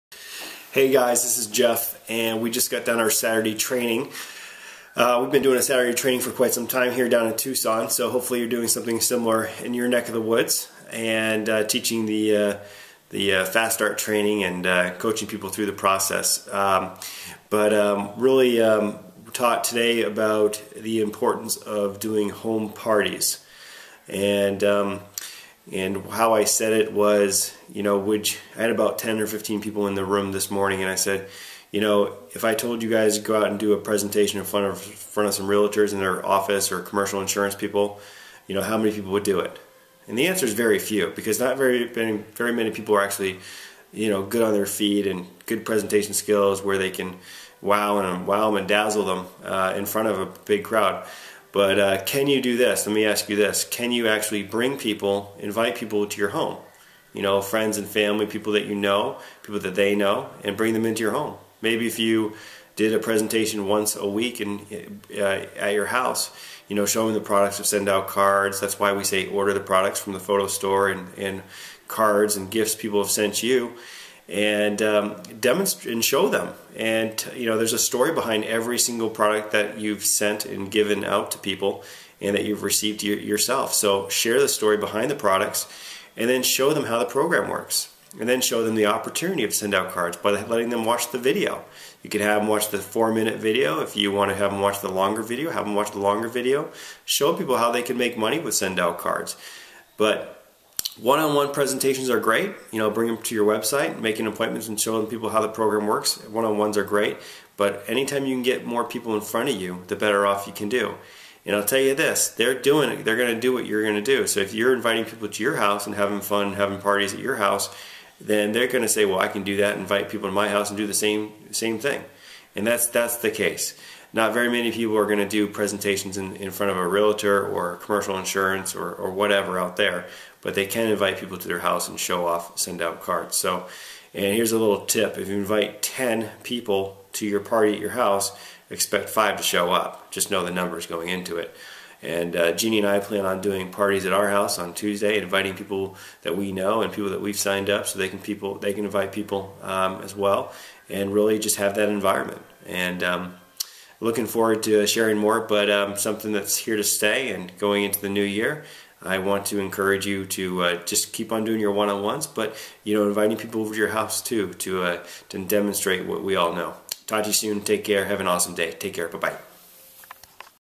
1 on 1's & home parties
doing one on ones is great! And keep on doing them...But inviting friends and new reps over to your house and demonstrating the power of Send Out Cards is even better.